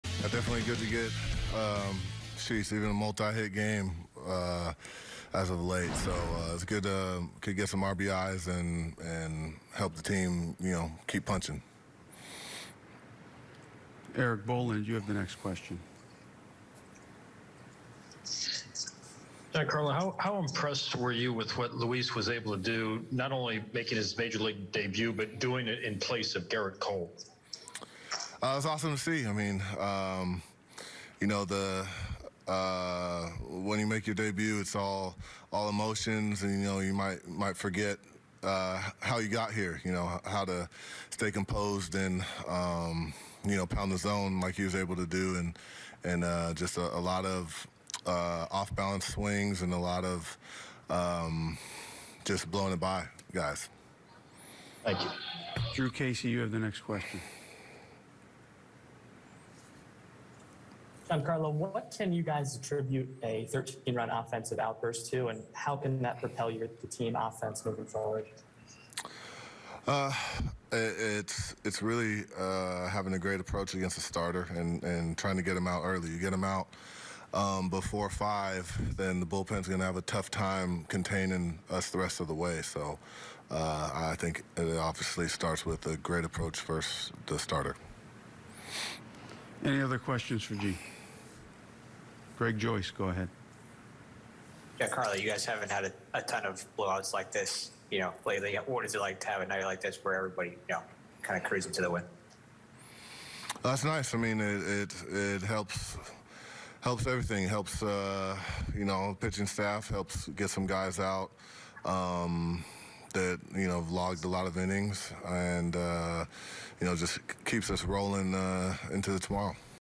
Giancarlo Stanton talks multi-hit game and speaks highly of rookie Luis Gil
Giancarlo Stanton talks multi-hit game and speaks highly of rookie Luis Gil after 13-1 win over the O's.